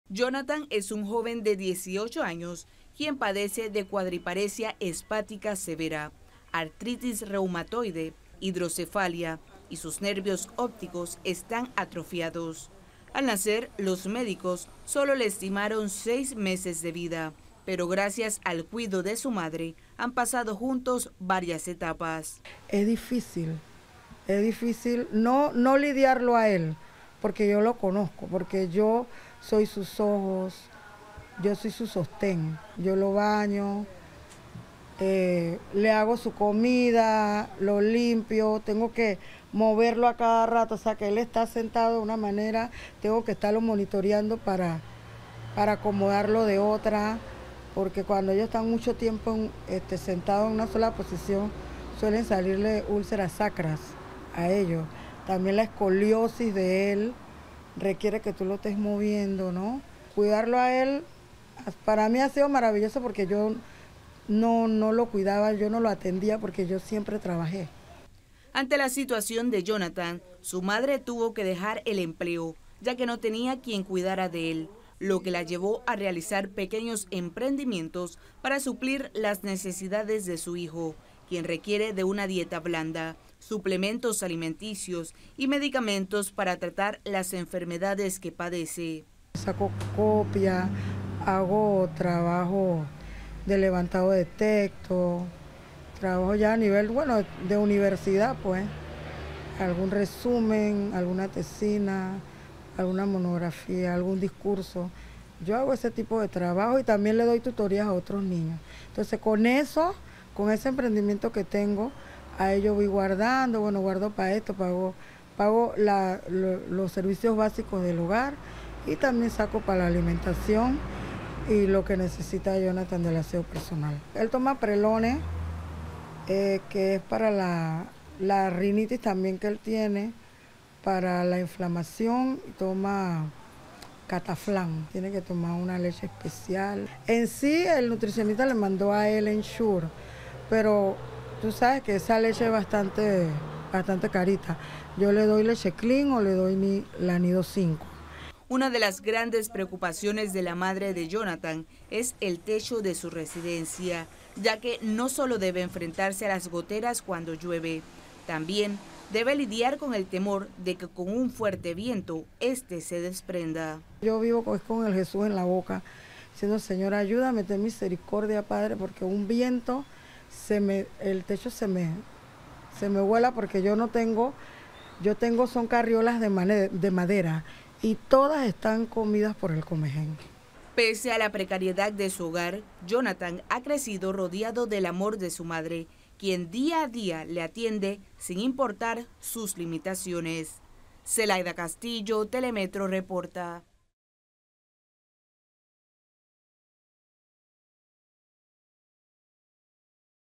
Jhonattan padece varias enfermedades, requiere ayuda para mejorar su calidad de vida
Una madre busca mejorar la calidad de su hijo en medio de las adversidades que ha tenido que enfrentar. Jhonattan padece varias enfermedades y requiere ayuda para adquirir medicamentos y leche especia.